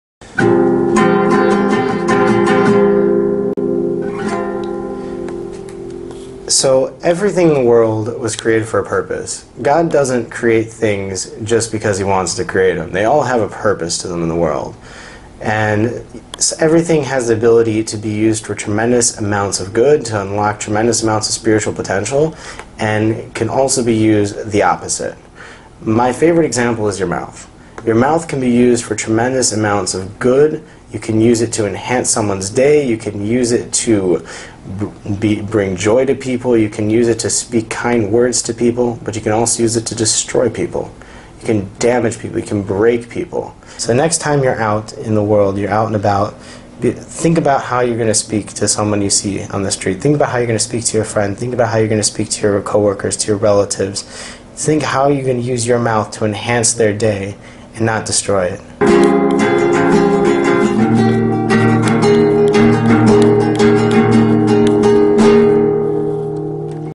How Will You Make An Impact? (60 Sec) Sean Littman
We were given a most amazing tool, our mouth, the way we use it impacts the world around us.
60 Seconds featuring Sean Littman